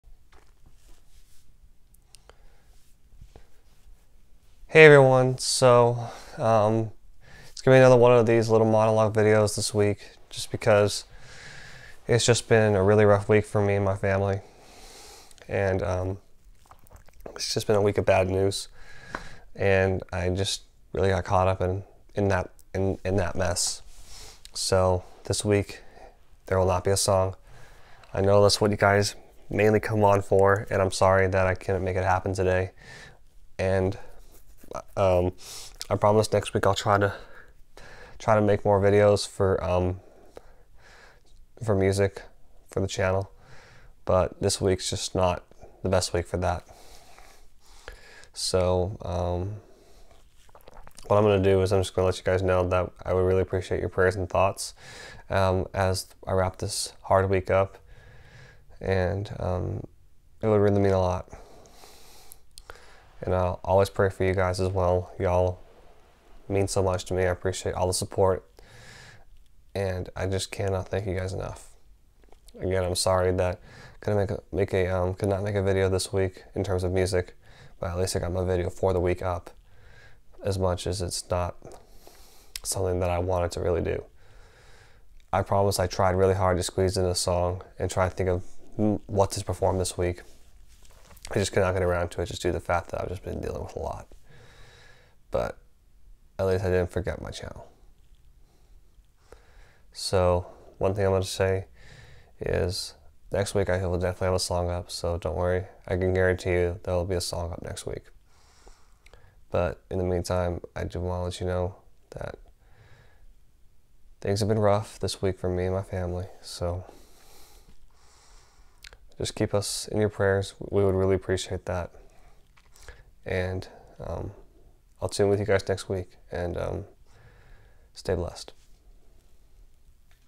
It's Been A Rough Week
Letting y'all know I've bee going through a burning hell this week so I will not be doing music but stay tuned for next weeks song cause I have something great planned out. And as always I appreciate all the continued support!